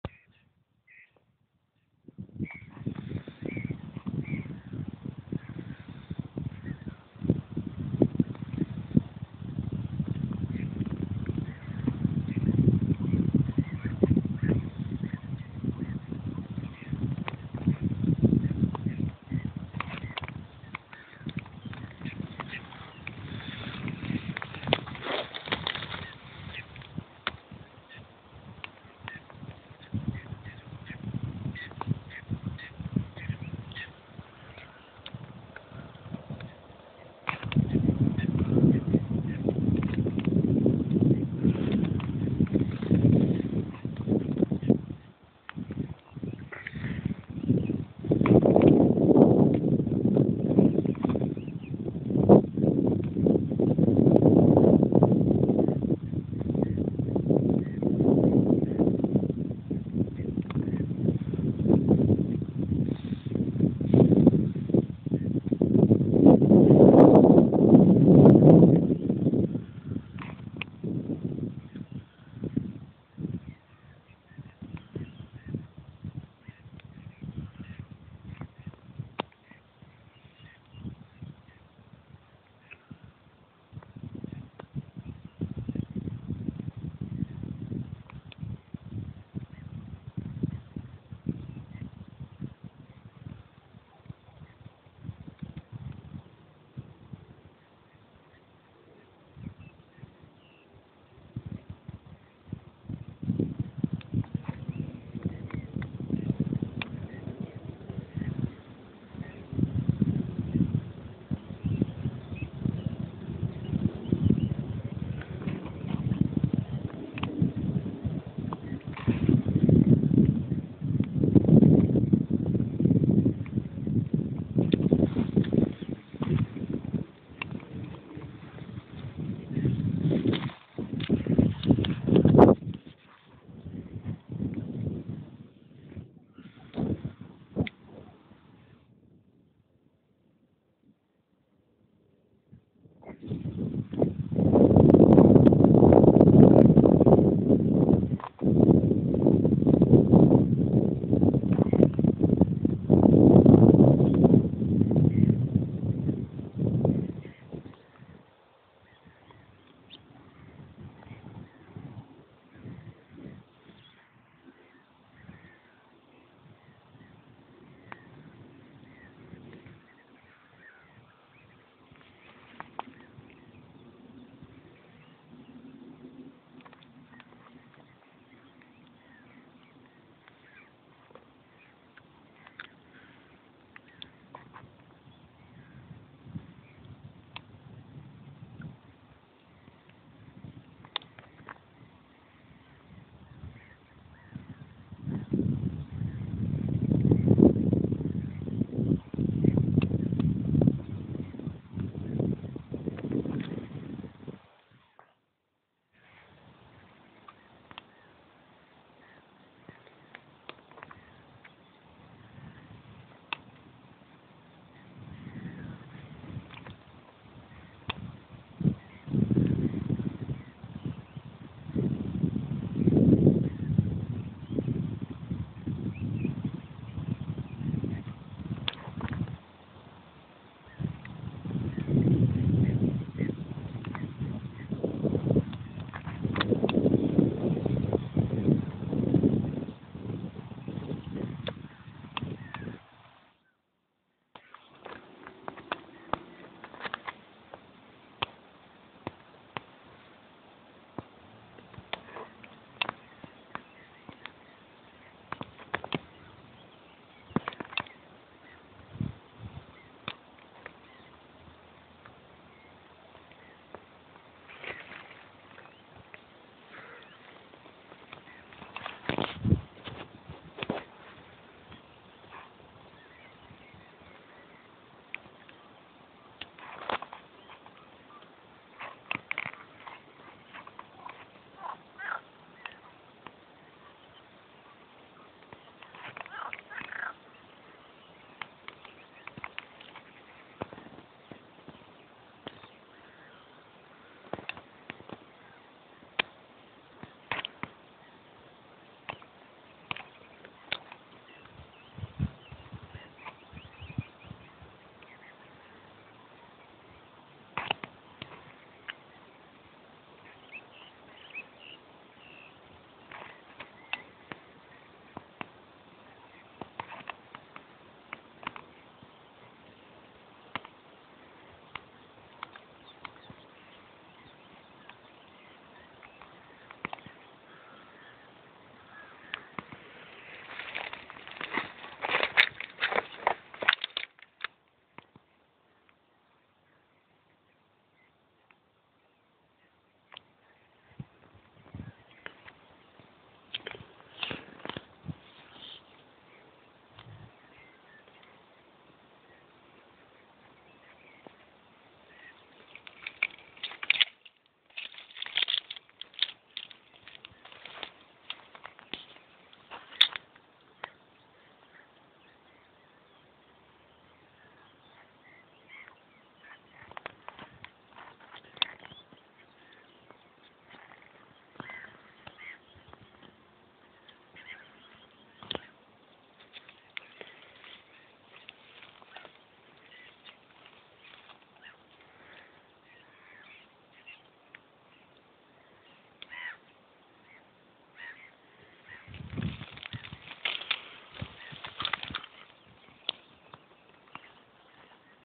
Jezioro Francuski Okop
Niedzielne wędkowanie na jeziorze "Francuski Okop" na łąkach w gminie Pomiechówek, w pobliżu ujścia rzeki Wkra do rzeki Narew